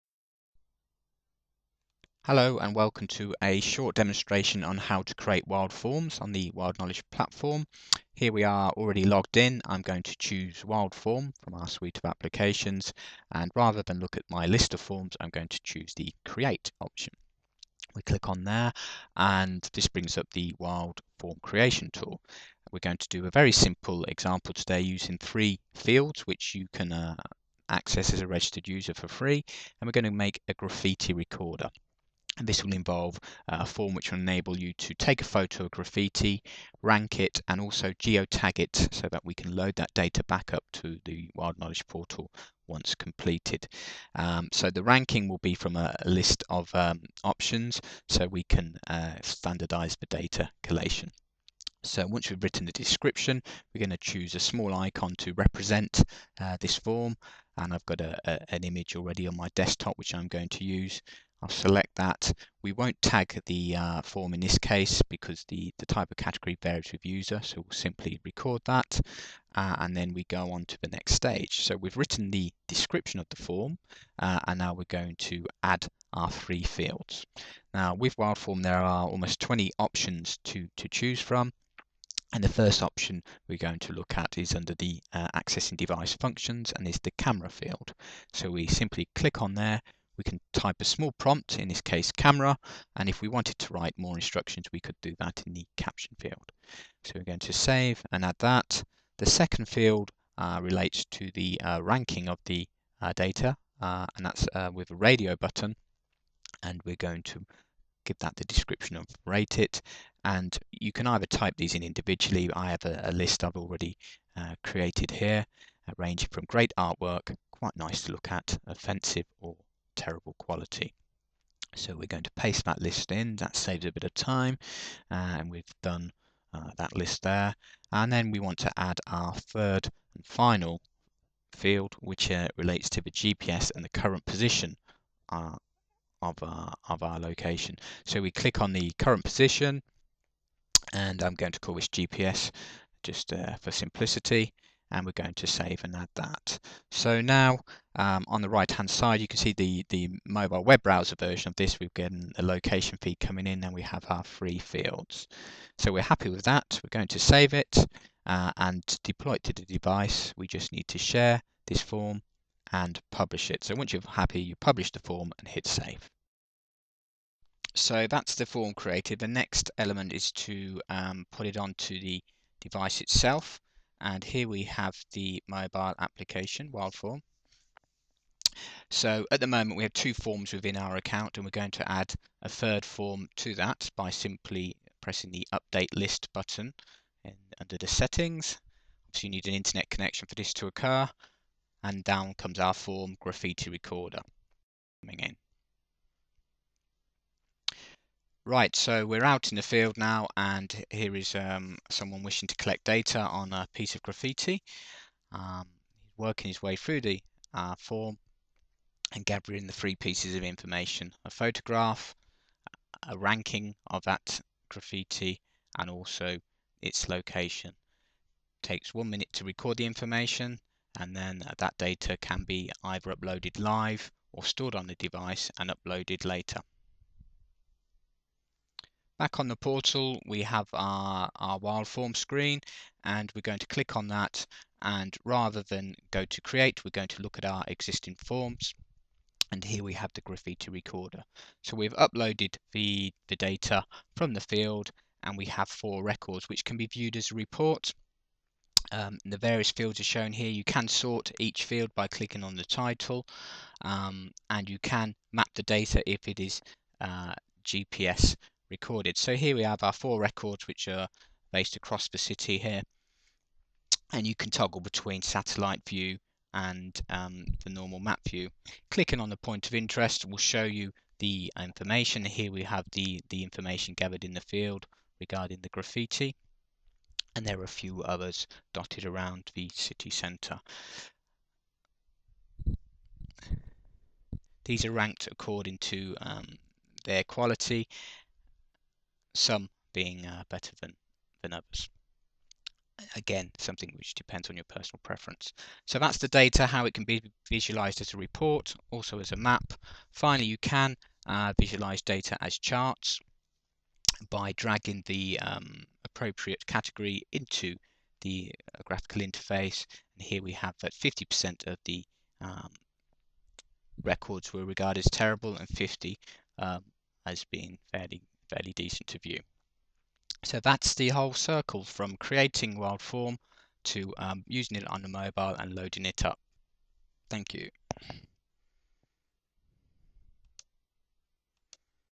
WildForm: How to create forms and gather data with your mobile
This short guide takes you through the 3 stages of using WildForm: 1) Create your form 2) Mobilise the form and gather data 3) Upload your data and visualise it as maps, charts and reports.